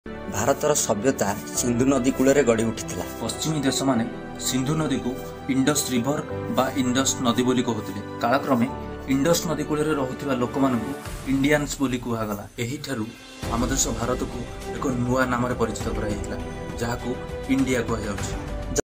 କାହିଁକି ଆମ ଦେଶର ନାମ ଭାରତ ହେଇଥିଲା ଓ କିଏ ଦେଇଥିଲେ ଏହି ନାମ
india backpacker vlogs
indian songs
news india
india bharat news